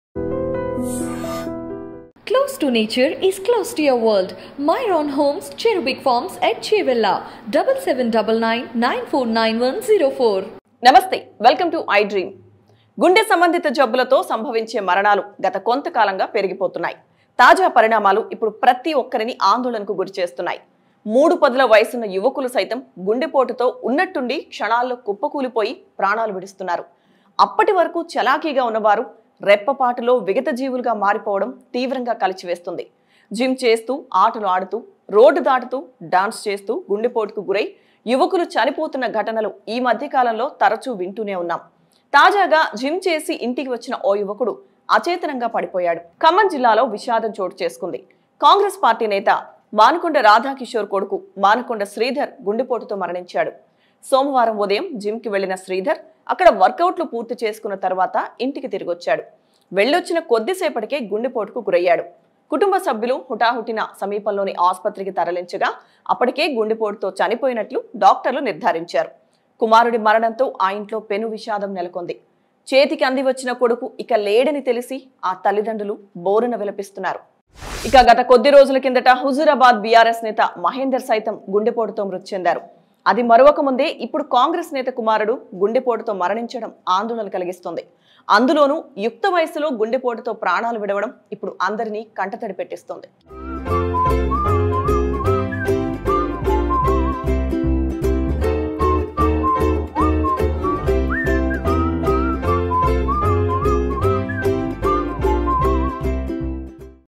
Congress Leader Radha Kishore Son Manukonda Sridhar Demise With Heart Attack | iDream
Congress Leader RadhaKishore Son ManukondaSridhar Demise With Heart Attack